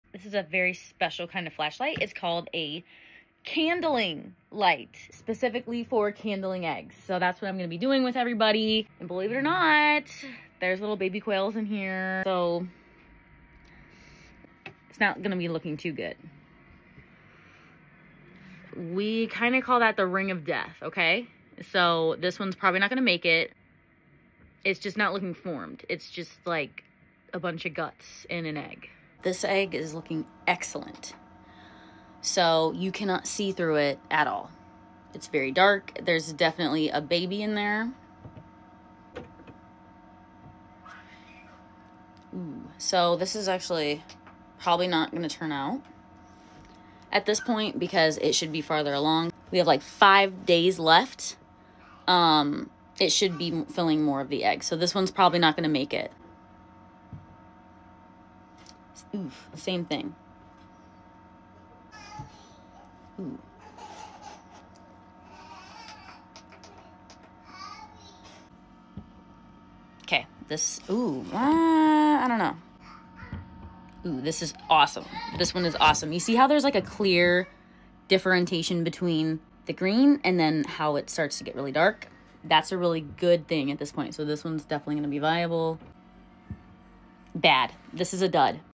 Candling Coturnix Quail Eggs: Day 14/18 incubation
What to expect and watch out for when candling incubated quail eggs